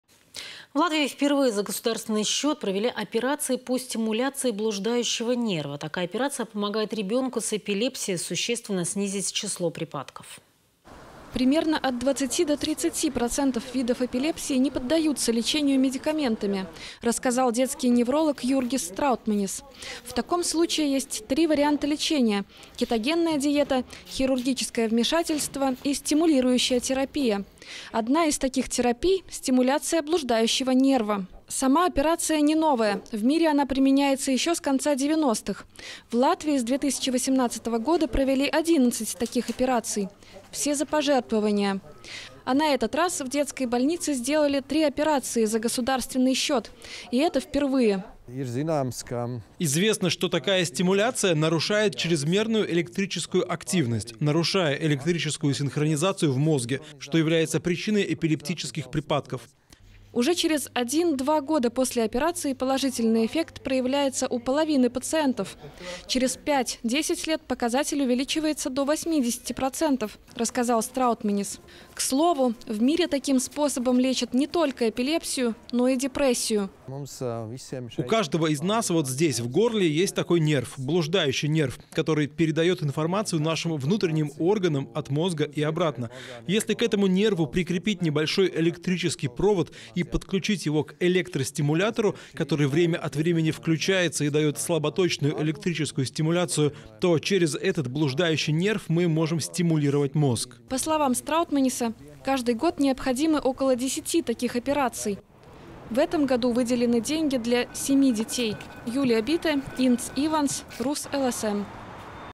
Бюджет оплатил первые операции по лечению детской эпилепсии
В Латвии впервые за государственный счёт провели операции по стимуляции блуждающего нерва. Такая операция помогает ребёнку с эпилепсией существенно снизить число припадков.